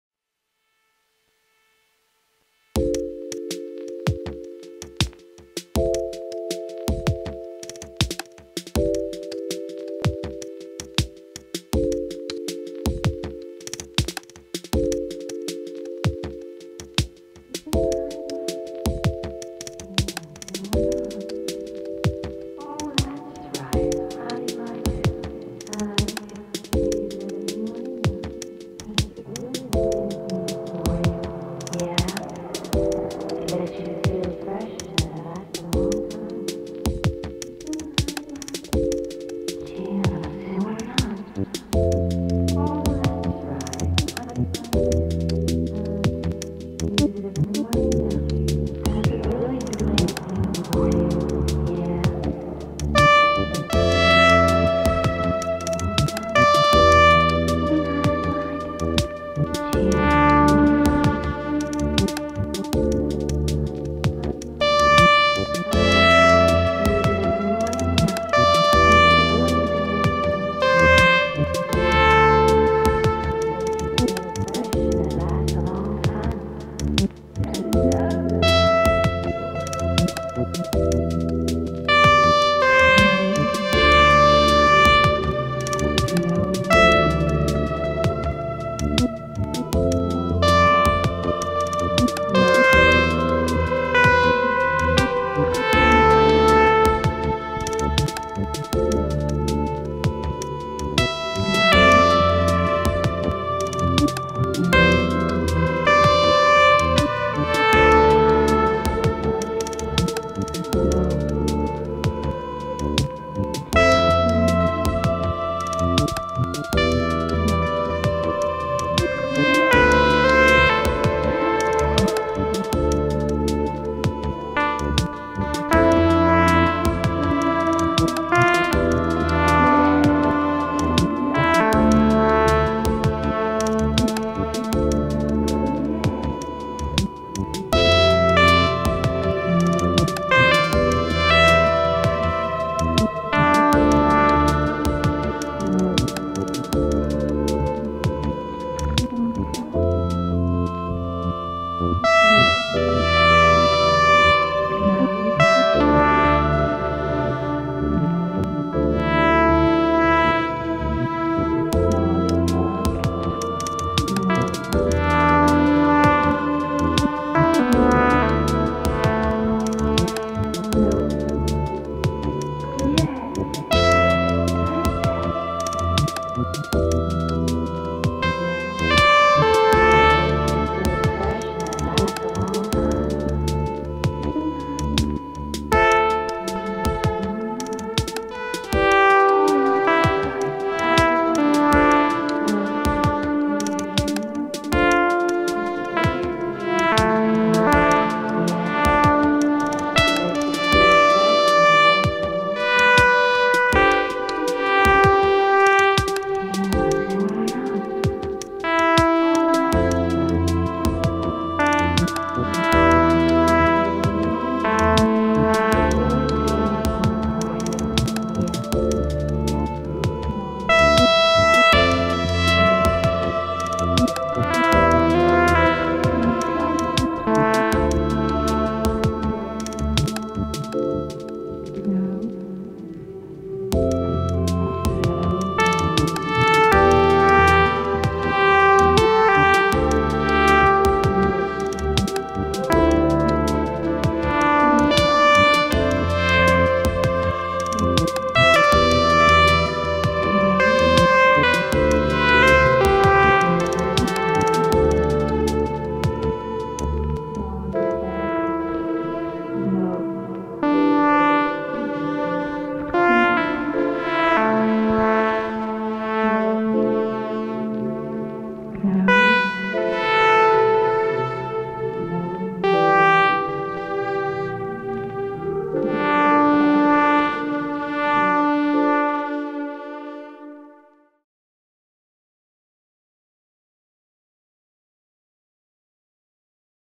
More Plastic. Teenage Engineering OP-Z meets modular synth via OP-lab
I recently got an teenage-engineering op-lab to finally connect the fantastic sequencer in the op-z directly to my modular via control voltage. Unfortunately it does not seem to support step components which reduces the fun by 90%. I hope a future firmware update will correct that.

For this one I am playing a lead DPO voice live. All drums, chords, bass are courtesy of the OP-Z - I am single patterns with the step components providing some variations. And I mute/un-mute various tracks throughout.

The voices/dialogue you hear in the background are played at reduced tape speed via the morpohagene and were recorded from some 70s TV clips.

I had a good time patching and recording this all in one day (holiday here).